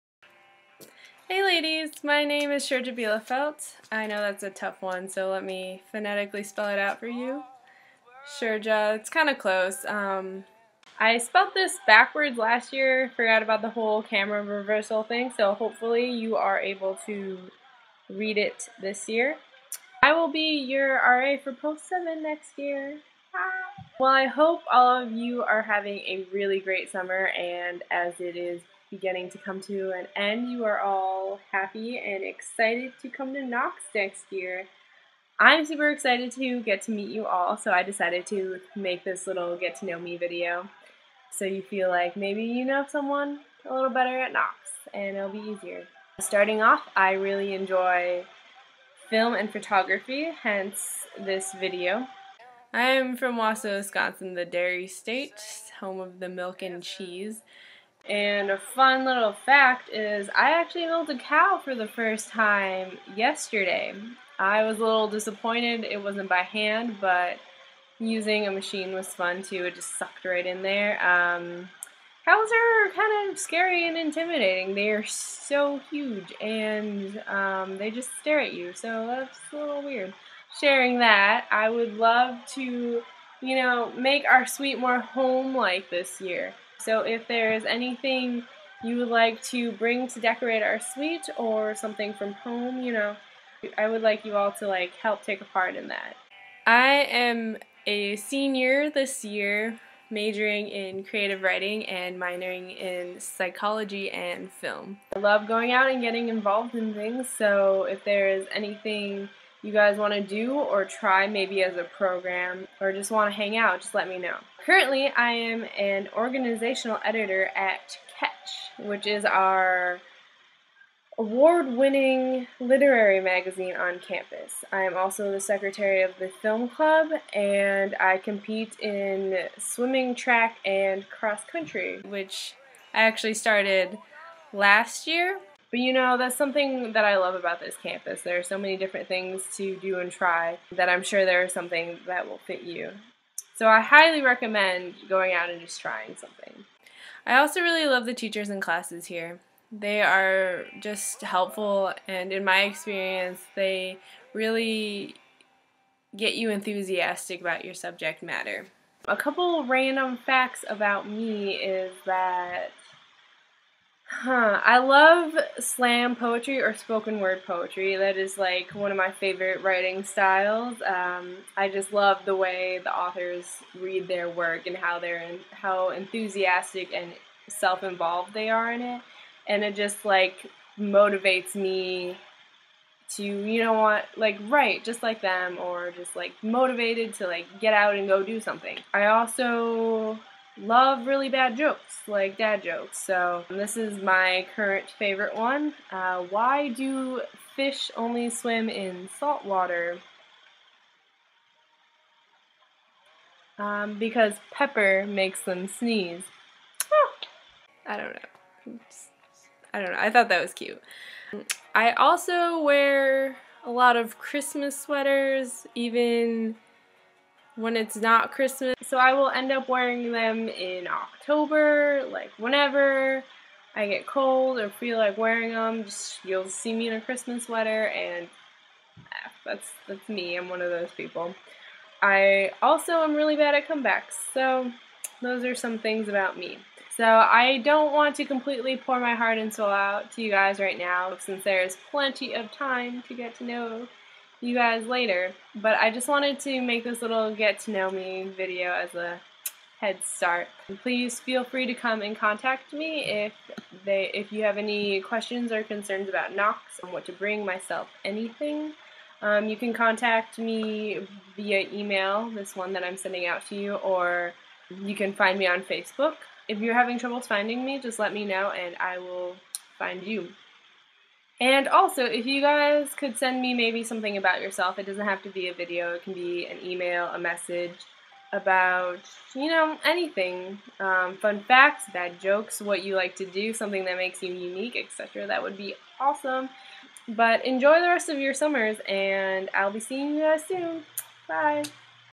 Post 7 RA Video
Getting pumped up!! Allow me to introduce myself, as RA of Post 7 :))